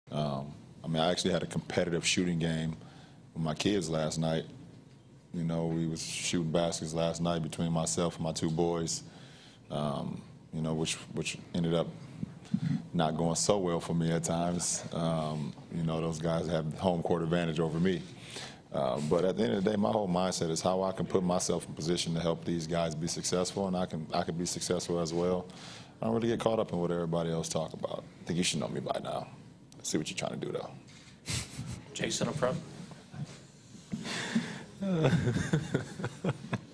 LeBron James prepared for Game 6 by shooting hoops with his kids
LeBron said he played basketball with his sons the night before.